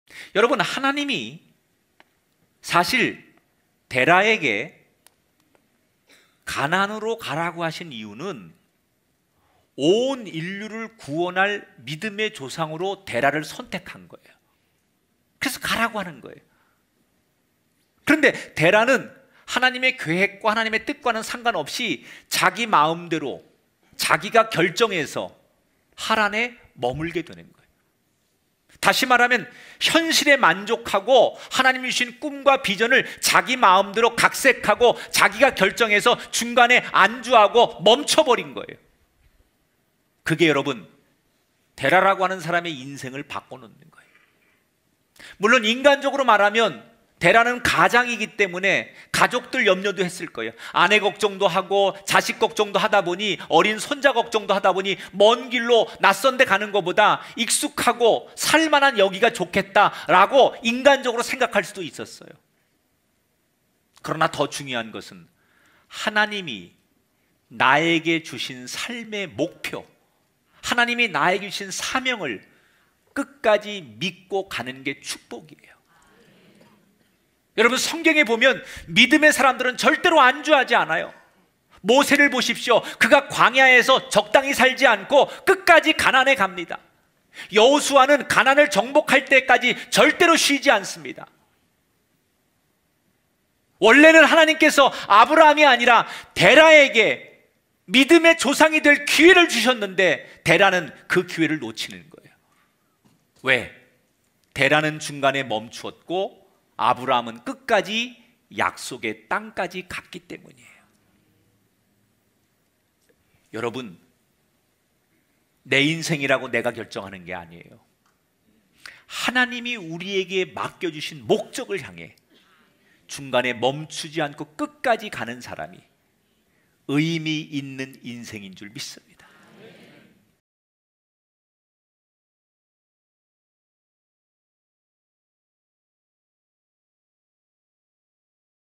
[짧은설교] 가나안으로 가라고 하신 이유 | 꿈의교회 김학중목사
" 밈음의 조상으로 선택하신겁니다 "

김학중 목사님의 전체 설교 영상이 궁금하신분은

🙏🏻오직 복음만을 전하는 생명의 말씀 채널! 'CTS2'
대한민국을 대표하는 차세대 목사님들의
은혜로운 주일설교를 다음 날 바로 보실 수 있습니다!